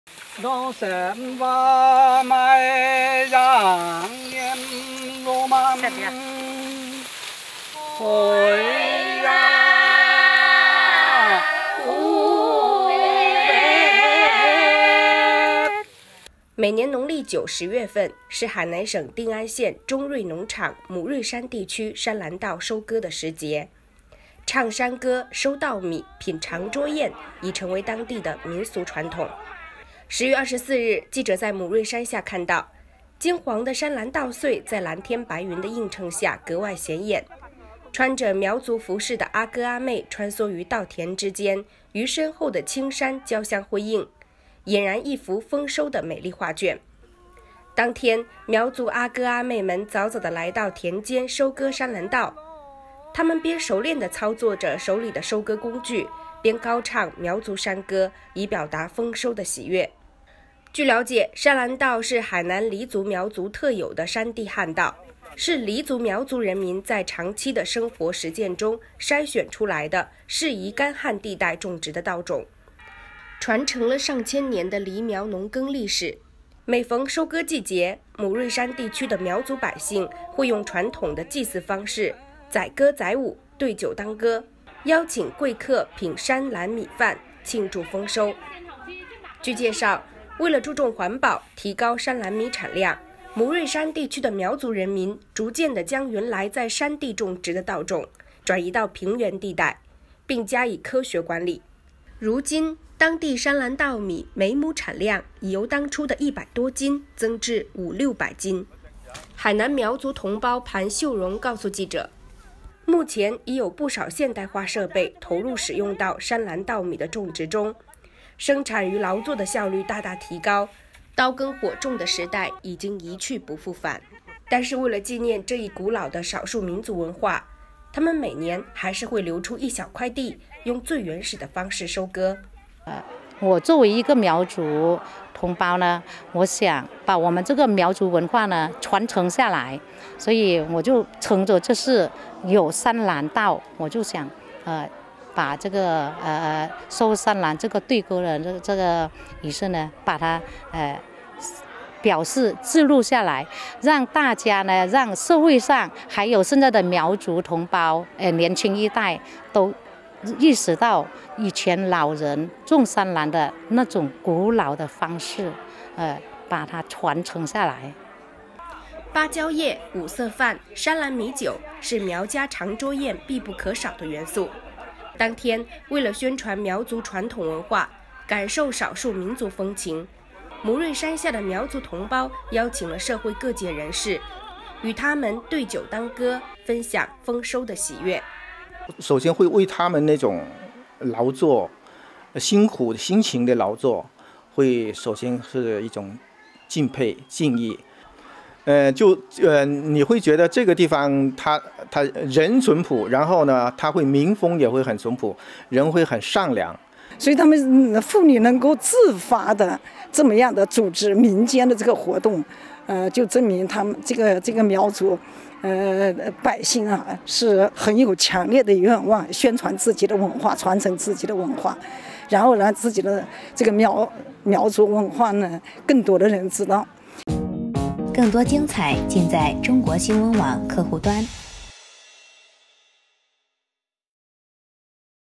唱山歌收山兰品盛宴 海南苗族百姓邀四方宾客庆丰收
农历九、十月份，又到了海南省定安县中瑞农场母瑞山地区山兰稻收割的时节。唱山歌、收稻米、品长桌宴，已成为当地的民俗传统。一起来看金黄的山兰稻穗，听苗族阿哥阿妹唱山歌，做苗族文化传承人~